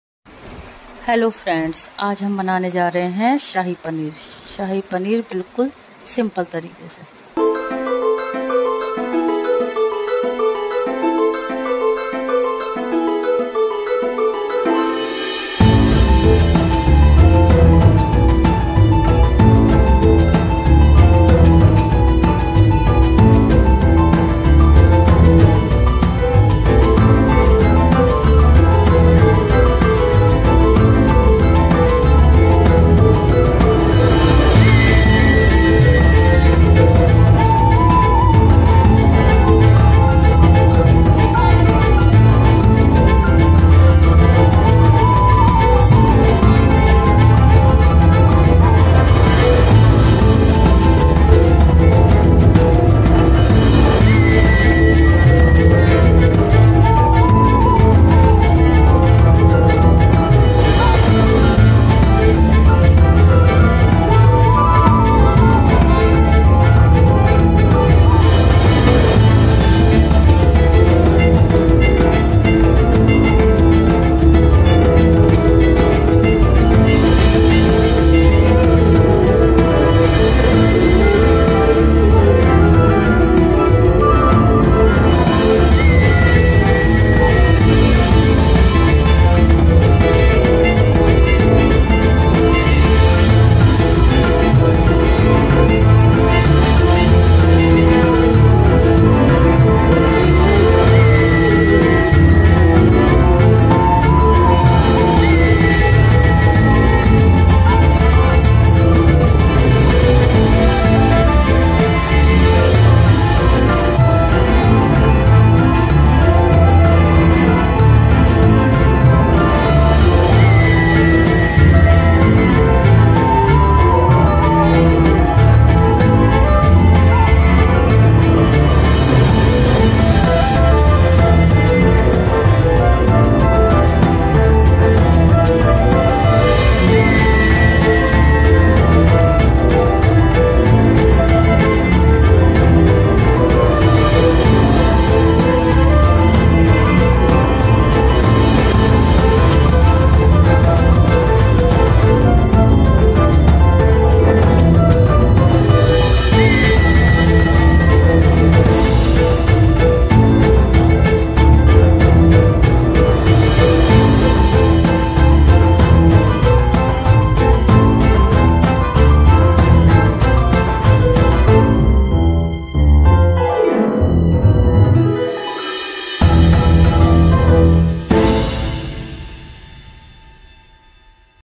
Less ingredients but full taste k sath bnaye Shahi paneer 😋😋😋😋🤤🤤
only 2 TSP oli and apne regular masale jaise haldi ,dhaniya , mirch ,namak k sath bnaye Shahi paneer jise bachhe s lekar busurg bhi aaram s kha sakte hai